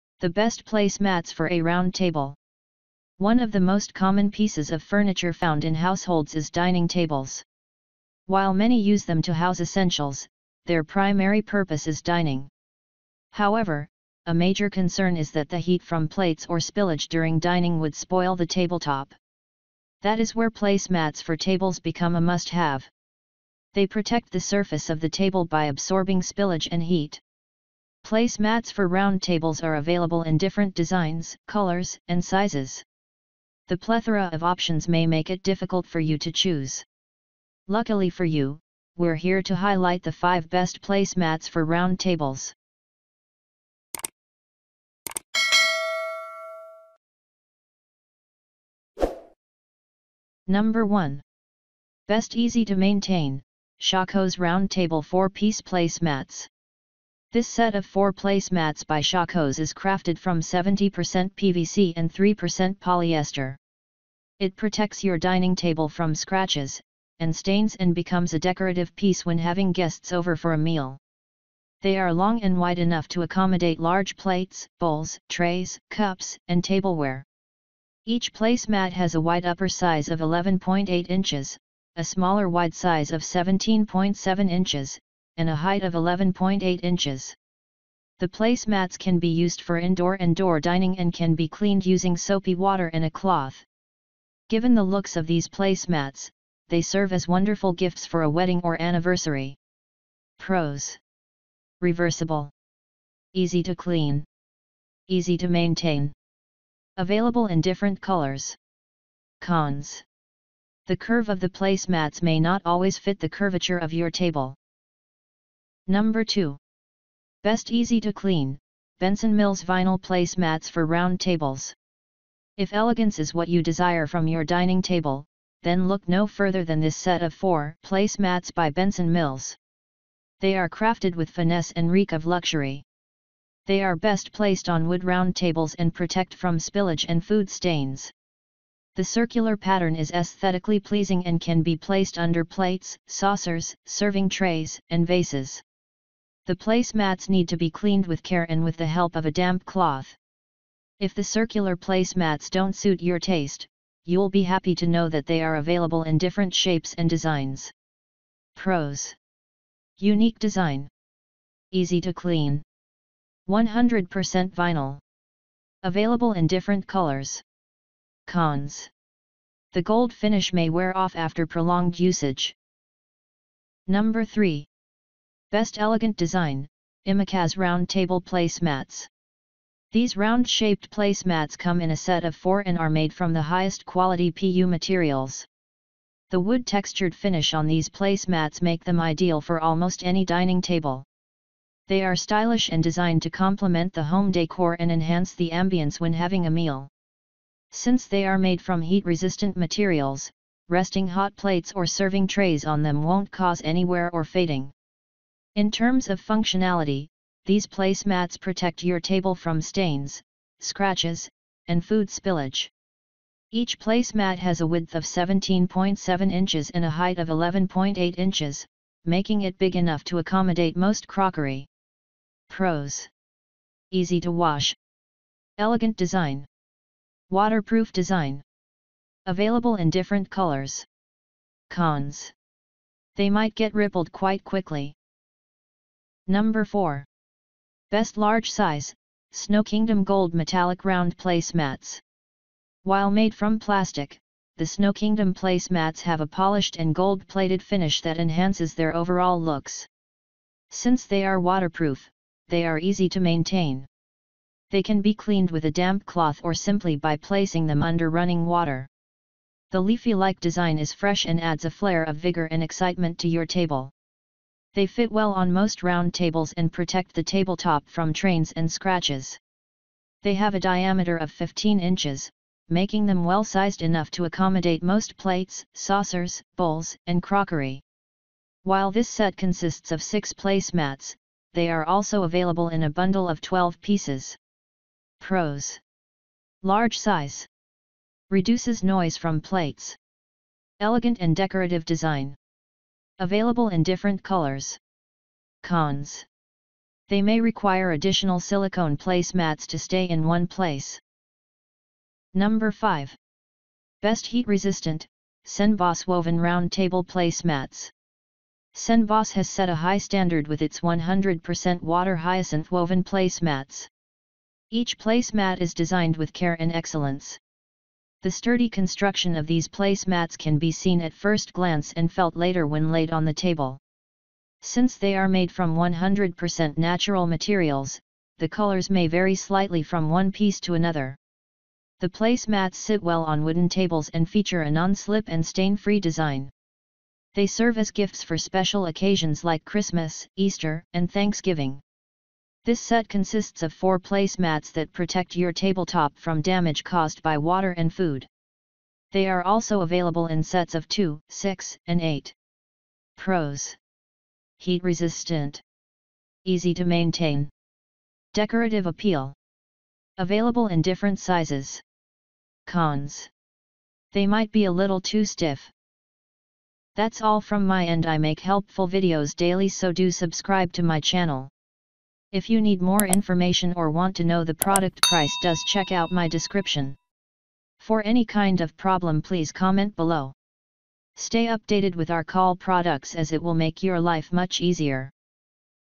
✅ Top 5 Best Placemats For A Round Table
👉1. SHACOS Round Table 4-Piece Placemats

👉2. Benson Mills Vinyl Placemats For Round Tables

👉3. IMMOKAZ Round Table Placemats

👉4. Snowkingdom Gold Metallic Round Placemats

👉5. CENBOSS Woven Round Table Placemats

 00:00 The Best Placemats For A Round Table.

One of the most common pieces of furniture found in households is dining tables. While many use them to house essentials, their primary purpose is dining.

However, a major concern is that the heat from plates or spillage during dining would spoil the tabletop.

That is where placemats for tables become a must-have. They protect the surface of the table by absorbing spillage and heat.

Placemats for round tables are available in different designs, colors, and sizes. The plethora of options may make it difficult for you to choose.

Luckily for you, we’re here to highlight the 5 best placemats for round tables.

  00:50 Number 1. Best Easy-To-Maintain: SHACOS Round Table 4-Piece Placemats.

This set of 4 placemats by SHACOS is crafted from 70% PVC and 3% polyester. It protects your dining table from scratches, and stains and becomes a decorative piece when having guests over for a meal.

They are long and wide enough to accommodate large plates, bowls, trays, cups, and tableware.

Each placemat has a wide upper size of 11.8 inches, a smaller wide size of 17.7 inches, and a height of 11.8 inches.

The placemats can be used for indoor and door dining and can be cleaned using soapy water and a cloth.

Given the looks of these placemats, they serve as wonderful gifts for a wedding or anniversary.

 01:55  Number 2.  Best Easy-To-Clean: Benson Mills Vinyl Placemats For Round Tables.

If elegance is what you desire from your dining table, then look no further than this set of 4 placemats by Benson Mills. They are crafted with finesse and reek of luxury. They are best placed on wood round tables and protect from spillage and food stains.

The circular pattern is aesthetically pleasing and can be placed under plates, saucers, serving trays, and vases. The placemats need to be cleaned with care and with the help of a damp cloth.

 02:54 Number 3.  Best Elegant Design: IMMOKAZ Round Table Placemats.

These round-shaped placemats come in a set of 4 and are made from the highest quality PU materials. The wood textured finish on these placemats make them ideal for almost any dining table.
They are stylish and designed to complement the home decor and enhance the ambiance when having a meal. Since they are made from heat-resistant materials, resting hot plates or serving trays on them won’t cause any wear or fading.

 03:56 Number 4.   Best Large Size: Snowkingdom Gold Metallic Round Placemats.

While made from plastic, the Snowkingdom placemats have a polished and gold-plated finish that enhances their overall looks. Since they are waterproof, they are easy to maintain. They can be cleaned with a damp cloth or simply by placing them under running water.

The leafy-like design is fresh and adds a flair of vigor and excitement to your table. They fit well on most round tables and protect the tabletop from trains and scratches.

 05:01  Number 5.   Best Heat-Resistant: CENBOSS Woven Round Table Placemats.

CENBOSS has set a high standard with its 100% water hyacinth woven placemats. Each placemat is designed with care and excellence.

The sturdy construction of these placemats can be seen at first glance and felt later when laid on the table. Since they are made from 100% natural materials, the colors may vary slightly from one piece to another.

You Can Buy it Right Now. We Made This List Based On Our Personal Opinion and hours of research & we have listed them based on the type of features and Price. We have included options for every type of user so whether you are looking for the best budget.